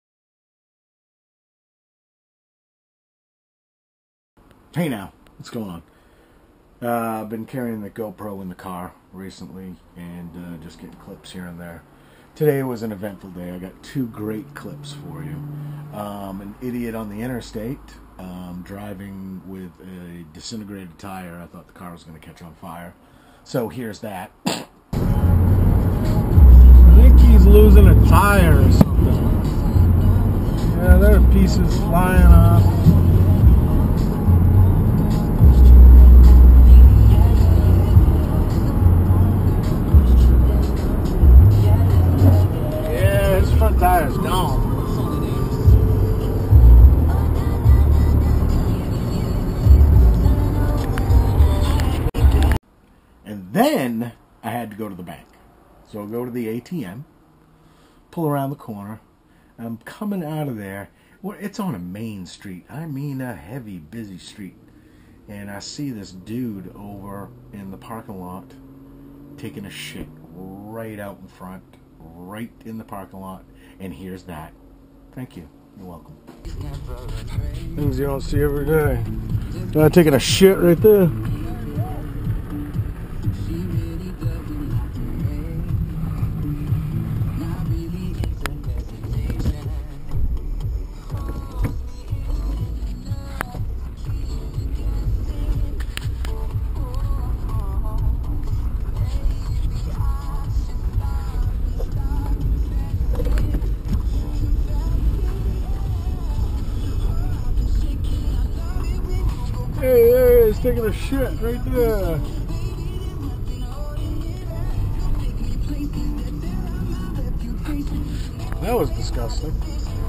Merry Shitmas
just some clips with the GoPro,oh an a guy shitting outside in the snow